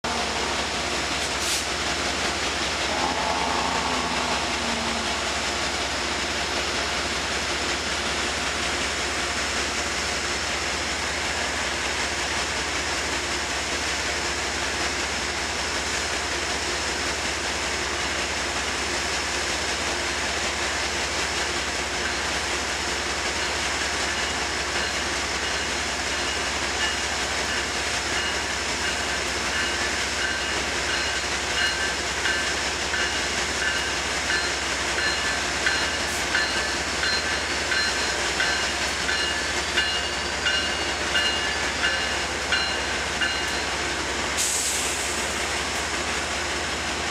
148 pulls in next to 7:43 danbury local
from yesterday. btw didn't get a vid of danbury leaving